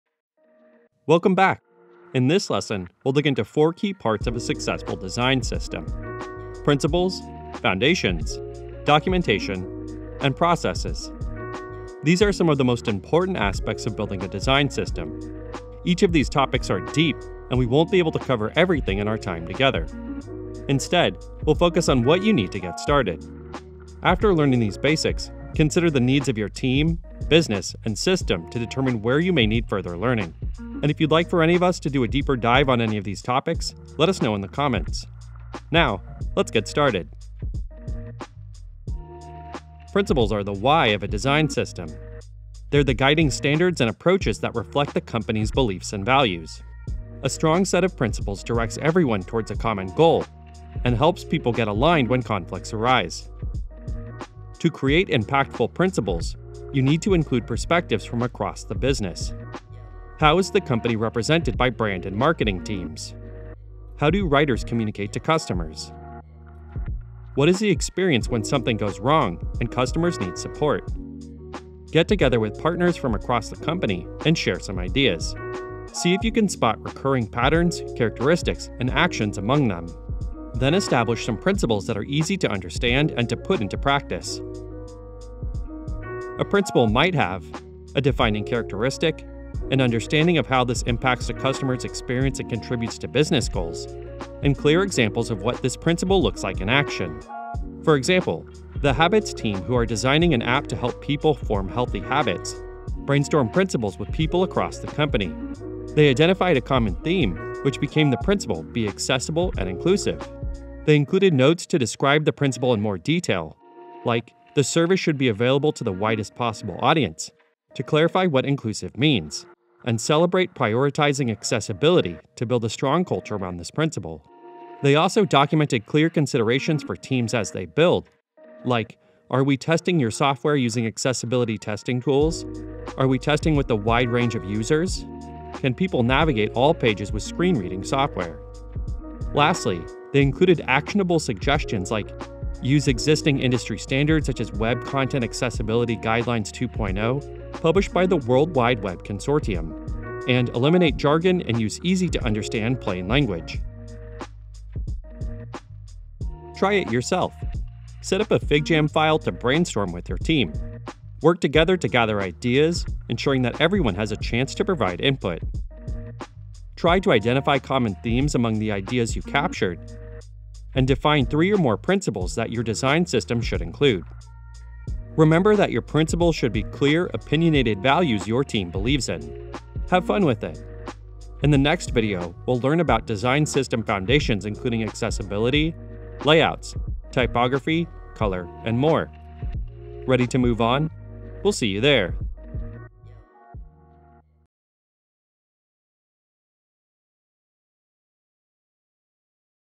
Define your design system's principles - Lesson 2 part 1 : Introduction to design systems
In this lesson, we’ll dig into four key parts of a successful design system.

1. Principles
2. Foundations
3. Documentation
4. Processes

00:00 - Introduction
00:37 - Principles
01:11 - Creating principles
01:25 - What a principle might have
02:35 - Try it yourself
03:00 - Wrap up